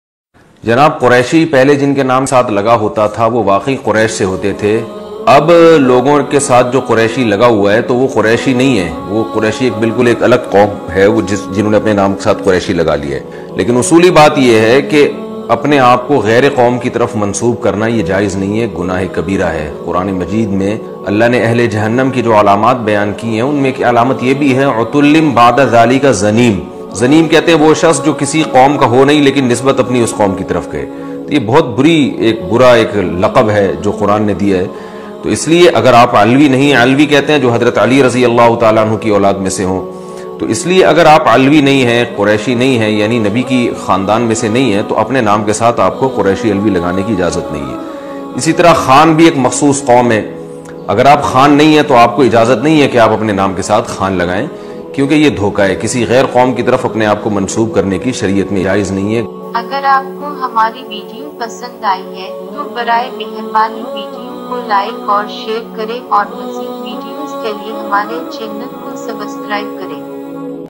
ᴀᴘɴɪ ǫᴏᴜᴍ ᴀᴜʀ ʜᴀsᴀʙ ɴᴀsᴀʙ ᴛᴀʙᴇᴇʟ ᴋᴀʀɴᴀ !
Hazart Muhammad SAW ki Hadess  ky Mutabiq ju log Ju log  jan bhouj  kar apana hasab nasb jan bhouj karty hain wu jannat main dakhil nahi hoon gy ,